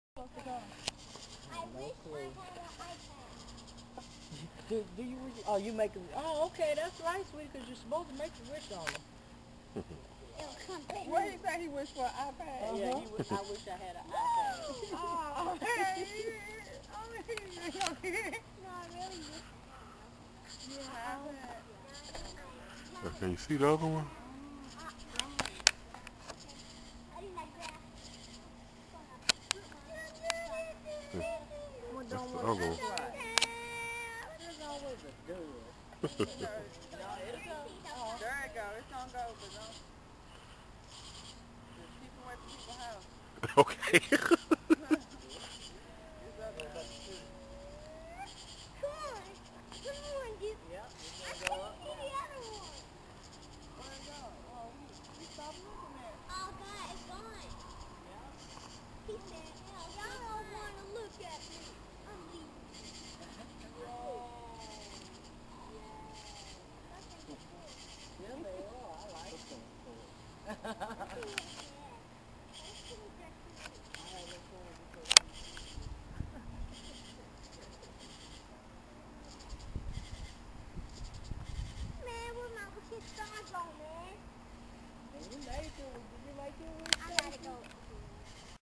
floating
Releasing floating candles for my girl Fay's fabulous 50th